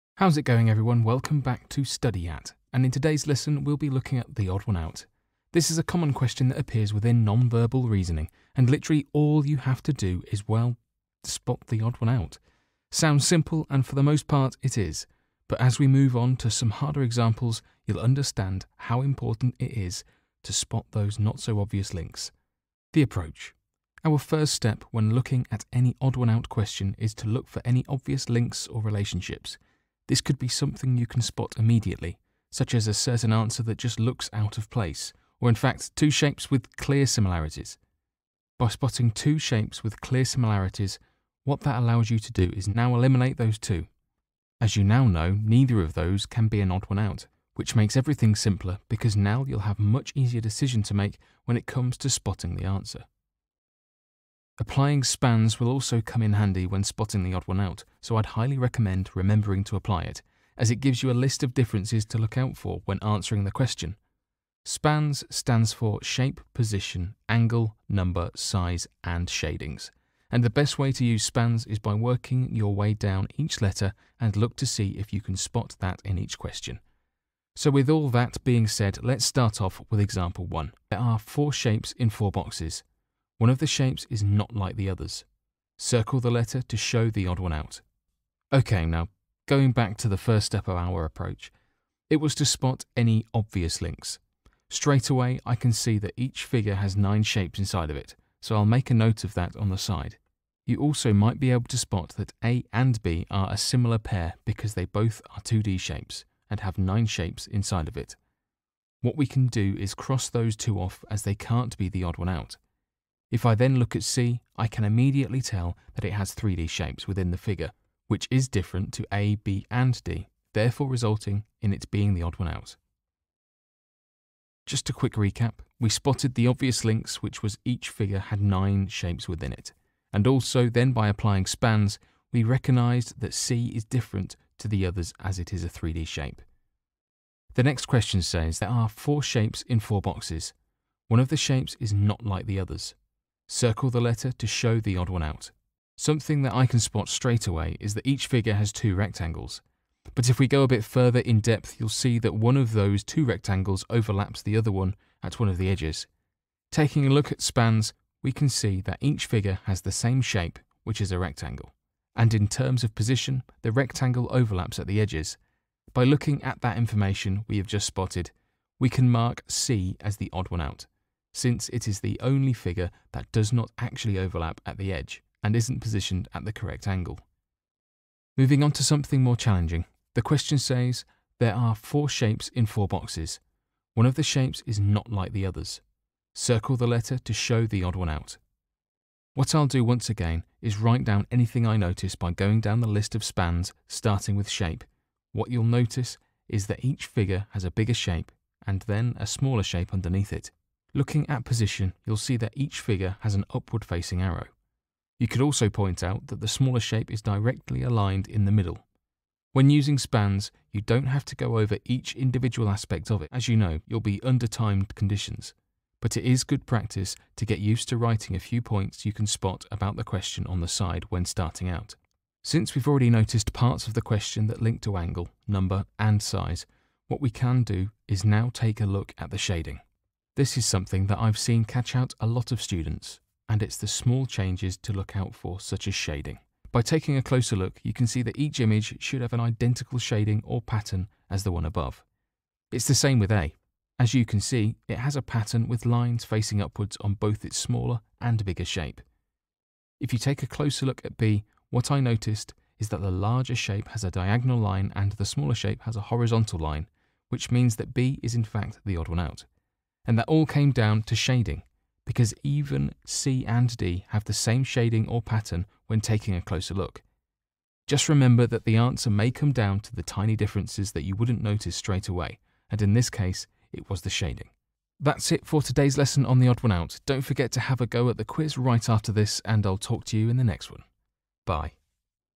Eleven Plus Non Verbal Reasoning - Odd One Out
In today's video we'll be going through the Odd One Out which is a topic of Non Verbal Reasoning that has frequently come up during the 11+ exam.